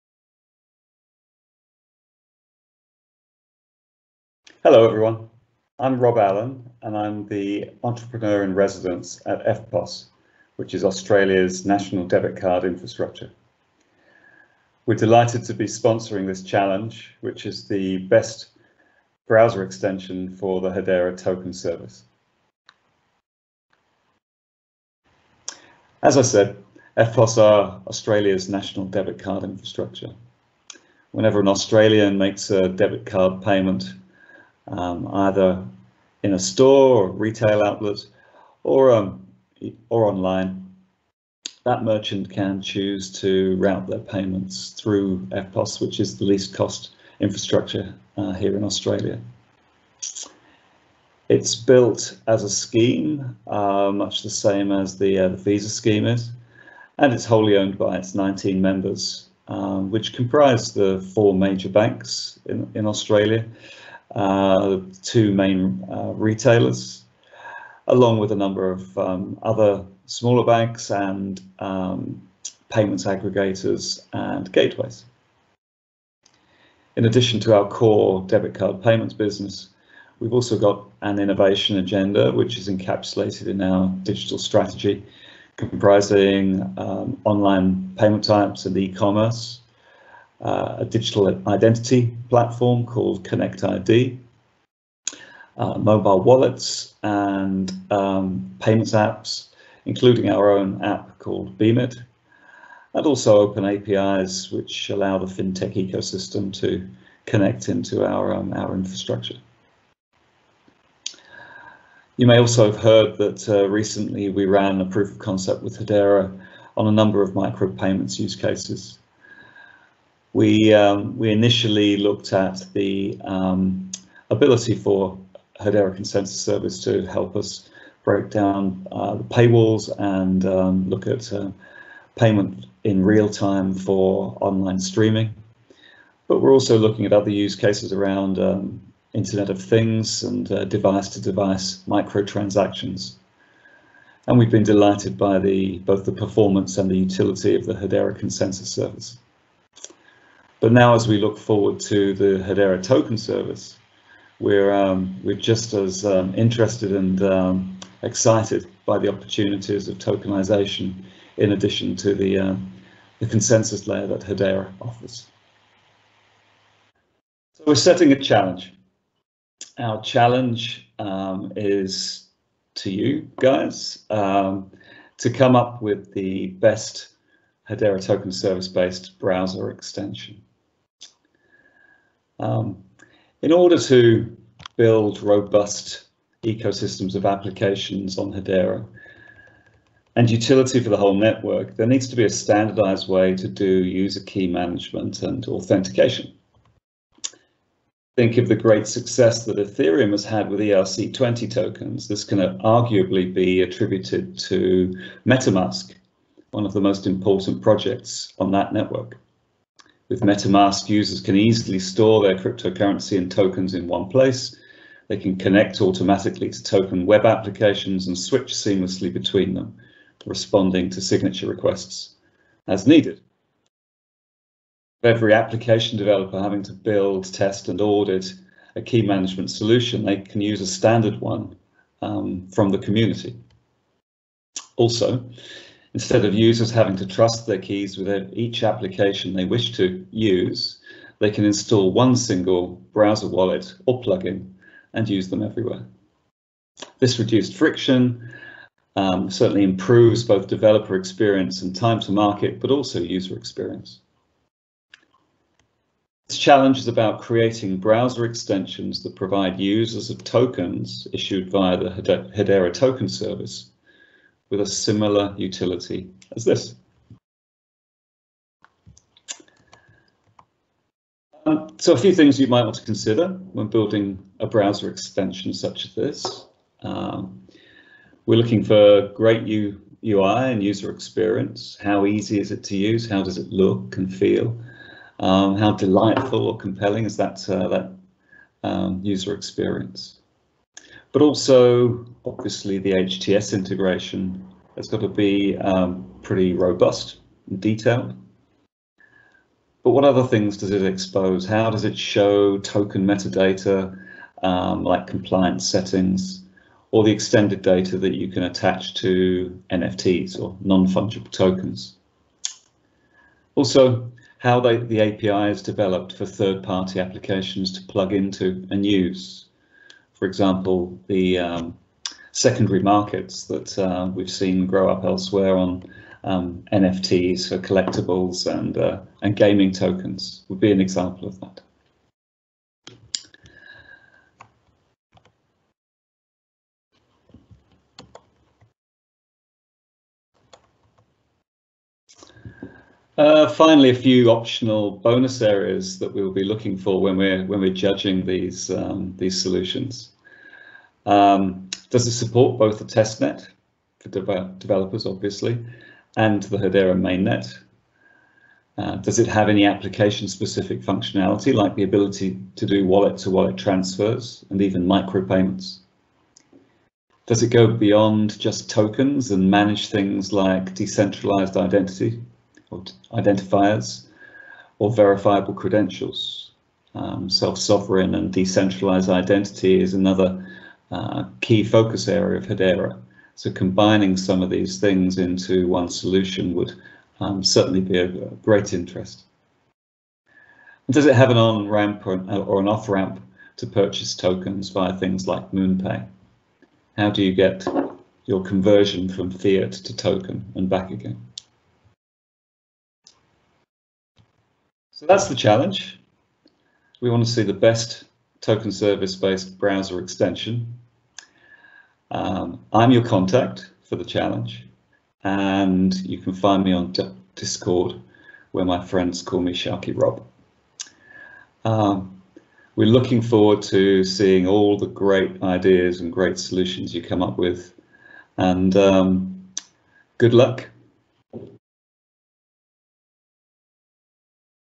Hedera21 Hackathon Challenge - Browser Extension sponsored by eftpos
eftpos Payments Australia is looking for the best HTS supported browser extension (e.g. the equivalent of Ethereum’s Metamask).

Prizes include:
•5,000 USD worth of hbar
•Business and technical mentorship by members of the Eftpos team

Find full details on all the challenges, judges, and prizes